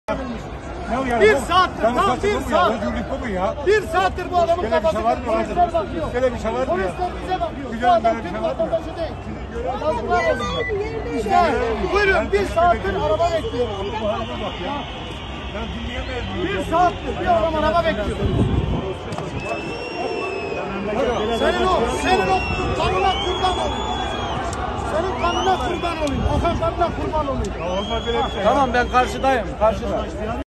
İmamoğlu'nun Erzurum mitingine saldırı
Erzurum'daki mitingine saldırı düzenlenen İmamoğlu, "Bu şehrin valisi, emniyet müdürü; biz de sizi seyrediyoruz. Devam edin. Erzurum Valisi, Erzurum Belediye Başkanı; hakkınızda suç duyurusunda bulunacağım" dedi.